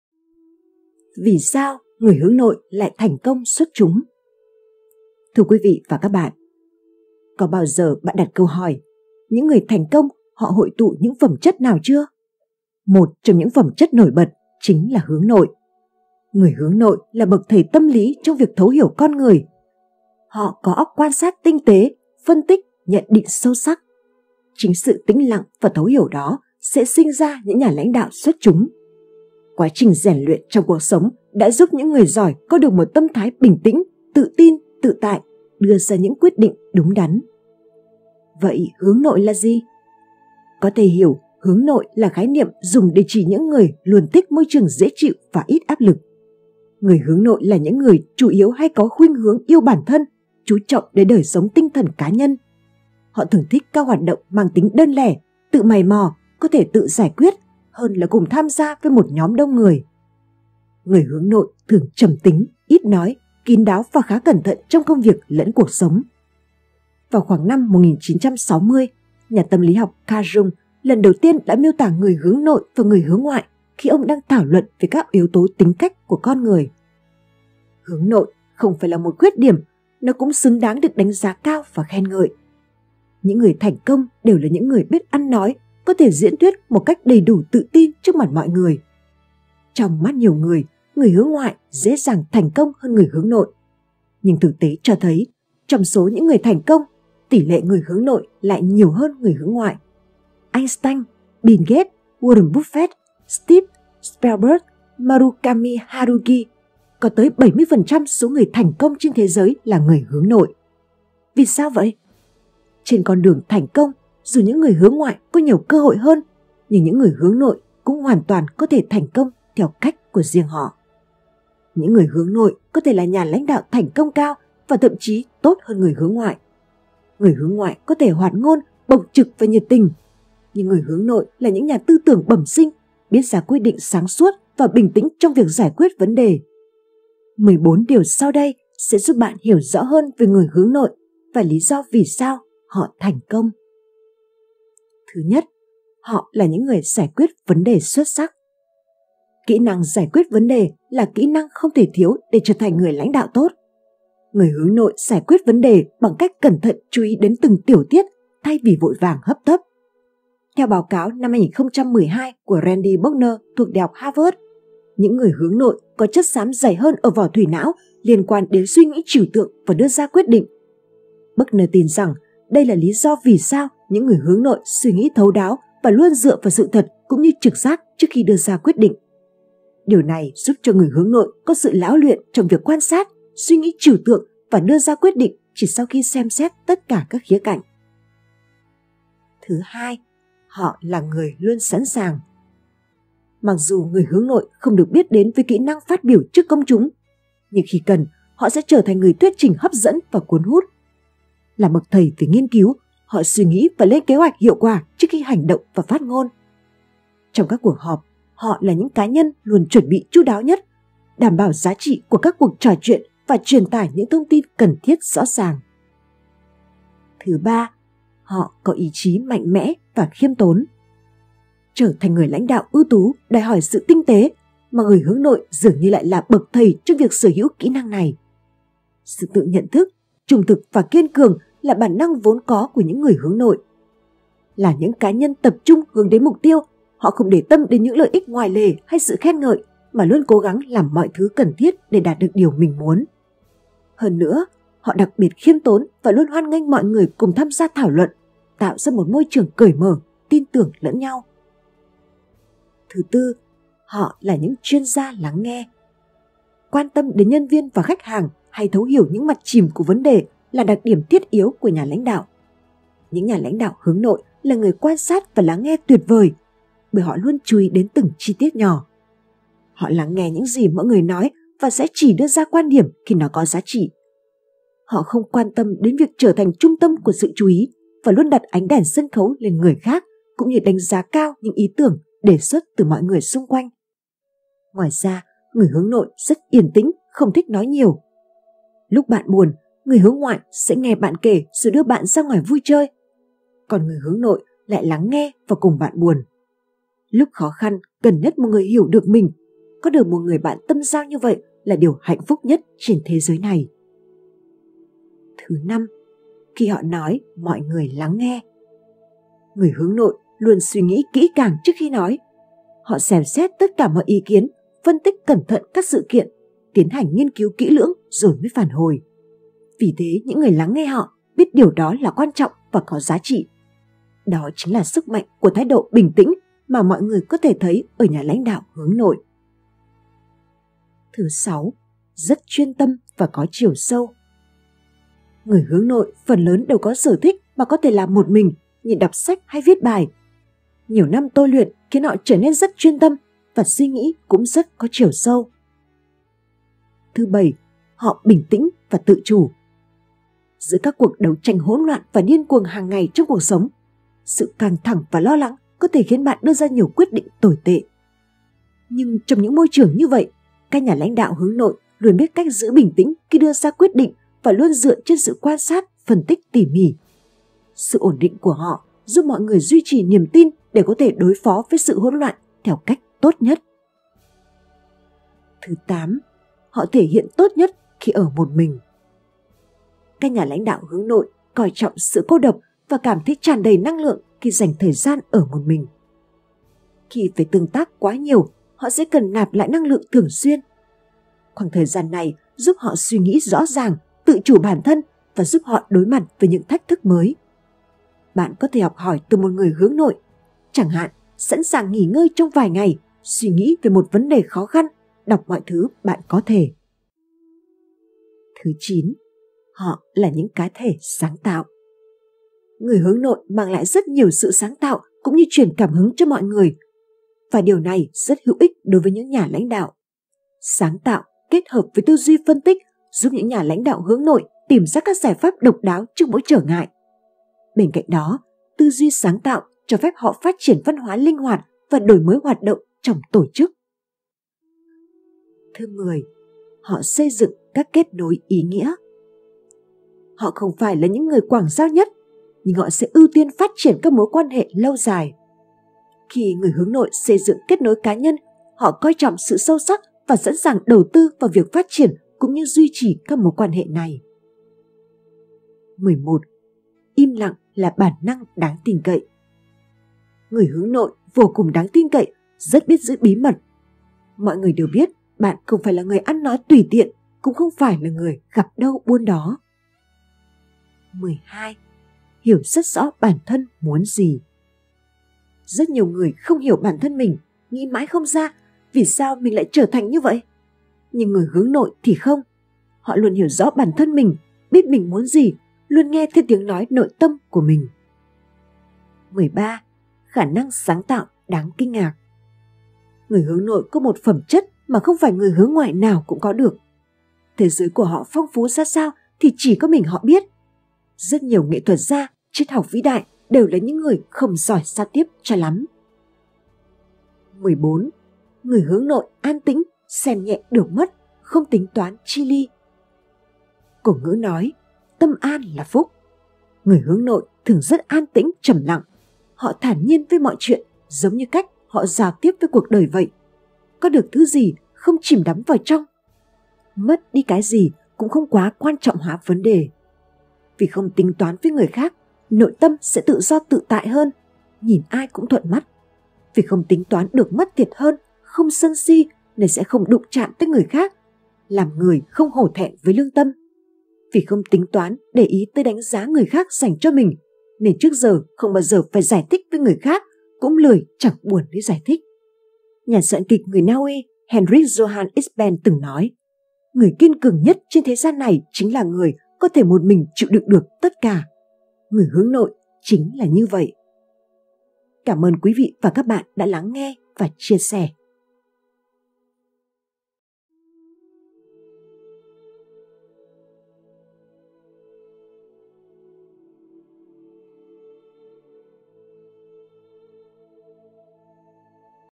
Vì Sao? Người Hướng Nội Lại Thành Công Xuất Chúng
Sách Tóm Tắt - Bí Quyết Thành Công
Admin: Kim Ngân

Video có thể có sử dụng nội dung chứa bản quyền dựa trên luật sử dụng hợp lý FairUse